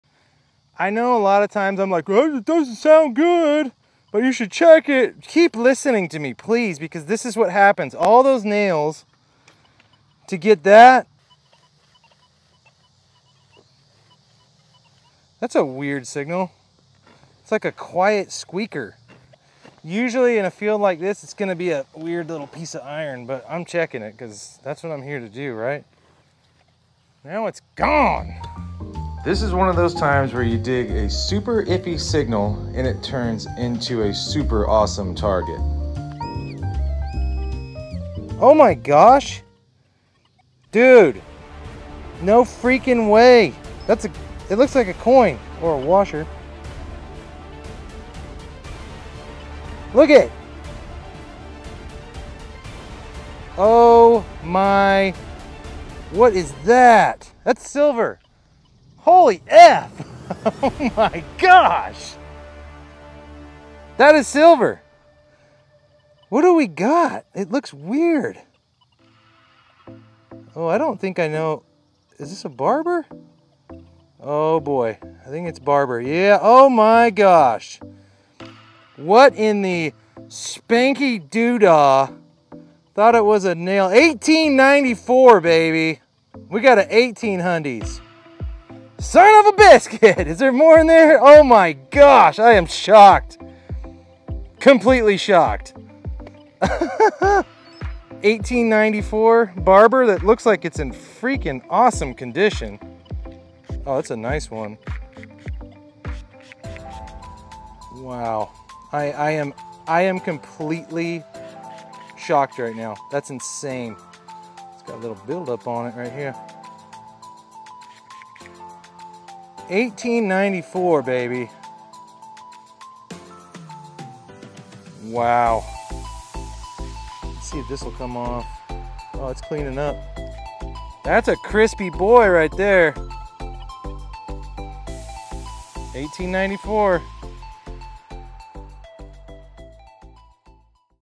This was nuts! Live action on TikTok!
I found this insanely nice coin while live streaming on TikTok! I really thought it would be trash, what a surprise!